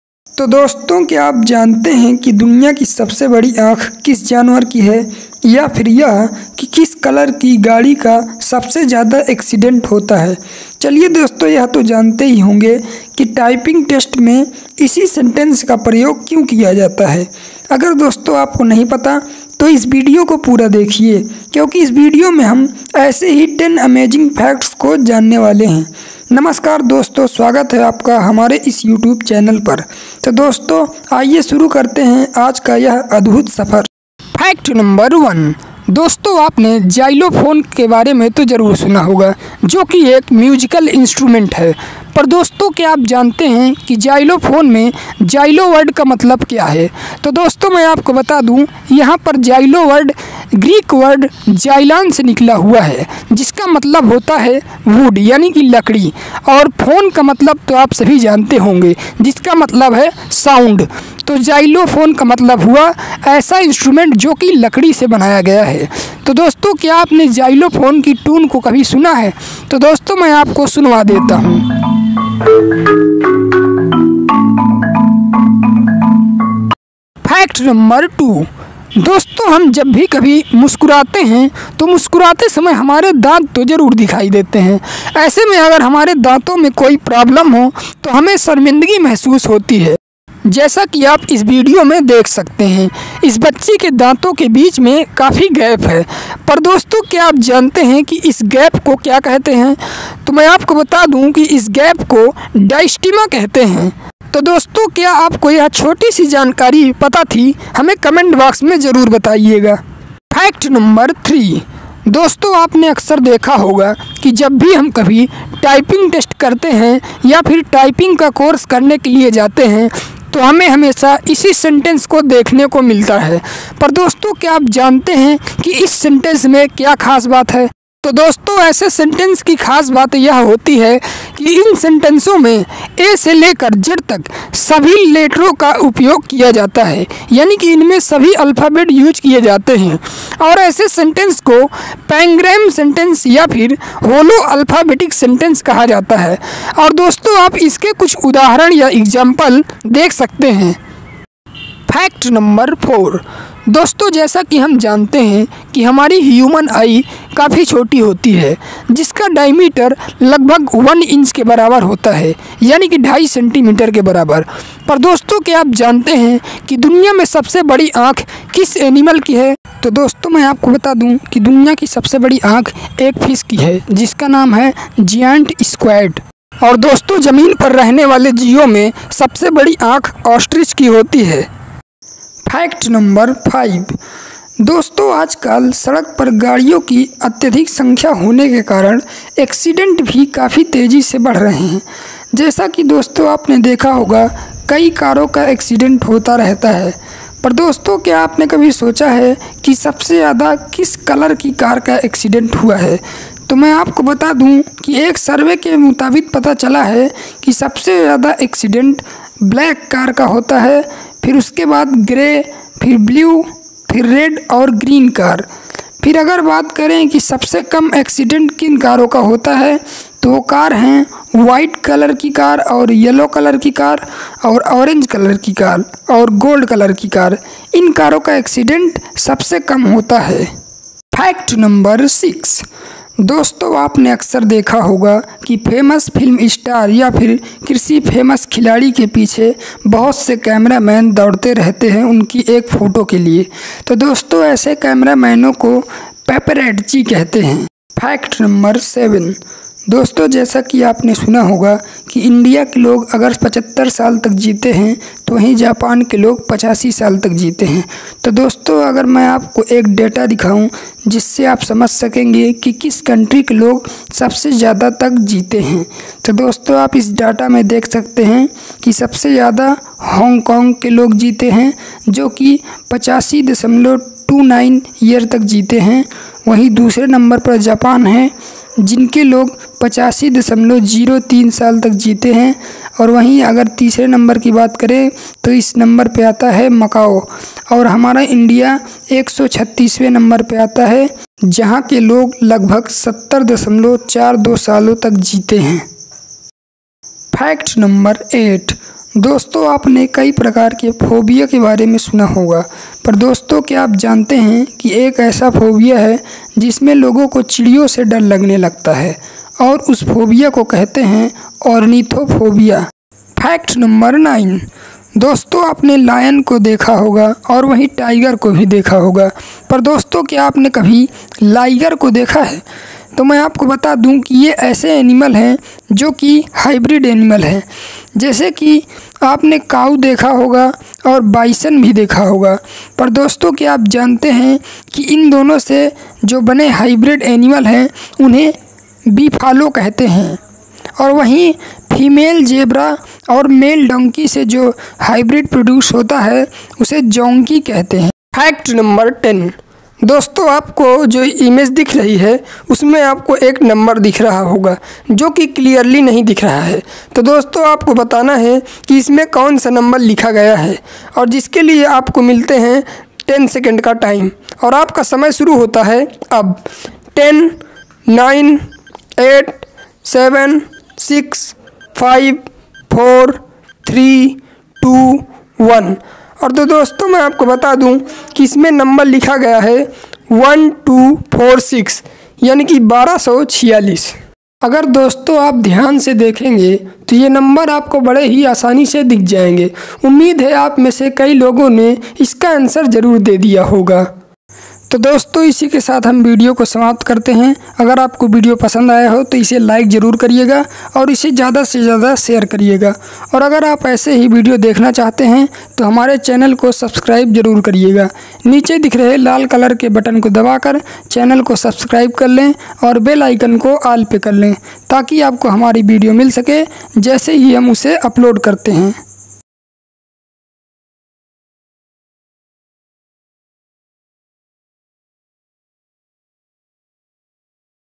Duniya mein Sabse badi aankh kis Janwar ki hai,दुनिया में सबसे बड़ी आंख किस जानवर कि है?largest eye.
In this Video we see a visual illusion image and 9 other facts.

Credit - all the images are downloaded from Google and Pexel.

Editor - Om Singh
Voice Over - Om Singh
Produced by - Vita app

Fact

1. in xylophone Xylo come from Greek word xylon which mean wood and sound mean voice or music.
2.the quick brown fox jumps over the lazy dog is a Pangram sentence or holoalphabetic sentence in which all the Alphabet are used.
3.Giant Squid have the largest eye in the animal kingdom.
4.did you know the safest car colour is white.
5.photographer who follow famous people for getting picture to sell them to a newspaper or magazine is called Paparazzi.
6.the life expectancy of Hong Kong is highest in the world.
7.Ornithophobia is the fear of Birds.
8.a cow bison hybrid is called beefalo and lion tiger hybrid is called liger.
9.Distema refer to a gap or space between the teeth.
10.this fact is a visual illusion in which a number is written not clear.